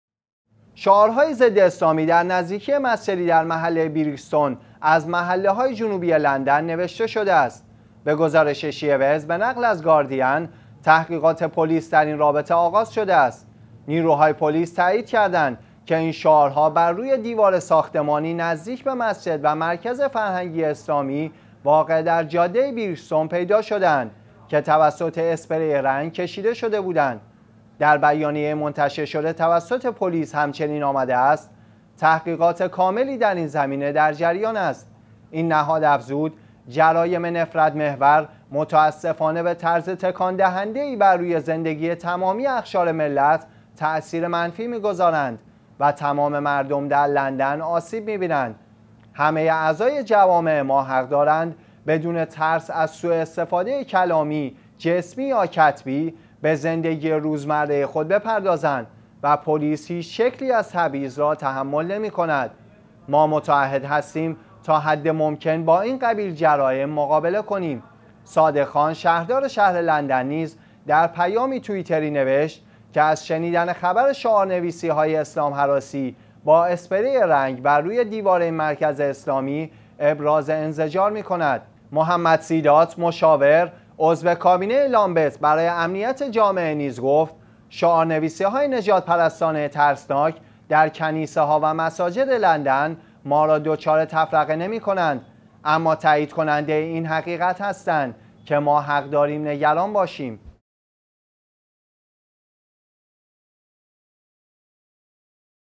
شعارنویسی ضداسلامی در مسجدی در جنوب لندن
شعارهای ضداسلامی در نزدیکی مسجدی در محله بریکستون از محله های جنوبی لندن نوشته شده است.
به گزارش شیعه ویوز به نقل از گاردین تحقیقات پلیس در این رابطه آغاز شده است.
نیروهای پلیس تایید کردند که این شعارها بر روی دیوار ساختمانی نزدیک به مسجد و مرکز فرهنگی اسلامی واقع در جاده بریکستون پیدا شده اند،که توسط اسپری رنگ کشیده شده بودند.
 در بیانیه منتشر شده توسط پلیس همچنین آمده است: تحقیقات کاملی در این زمنیه در جریان است.
 این نهاد افزود: جرایم نفرت محور متاسفانه به طرز تکان دهنده ای بر روی زندگی تمامی  اقشار ملت تاثیر منفی می گذارند و تمام مردم در لندن آسیب می بینند.
همه اعضای جوامع ما حق دارند بدون ترس از سوء استفاده کلامی، جسمی یا کتبی، به زندگی روزمره خود بپردازند و پلیس  هیچ شکلی از تبعیض را تحمل نمی کند؛ ما متعهد هستیم تا حد ممکن با این قبیل جرایم را مقابله کنیم.
صادق خان شهردار شهر لندن نیز در پیامی توییتری نوشت که از شنیدن خبر شعارنویسی های اسلام هراسی با اسپری رنگ بر روی این مرکز اسلامی ابراز انزجار می کند.
محمد سیدات، مشاور، عضو کابینه لامبت برای امنیت جامعه نیز گفت: شعارنویسی های نژاد پرستانه ترسناک در کنیسه ها و مساجد لندن ما را دچار تفرقه نمی کنند، اما تایید کننده این حقیقت هستند که ما حق داریم نگران باشیم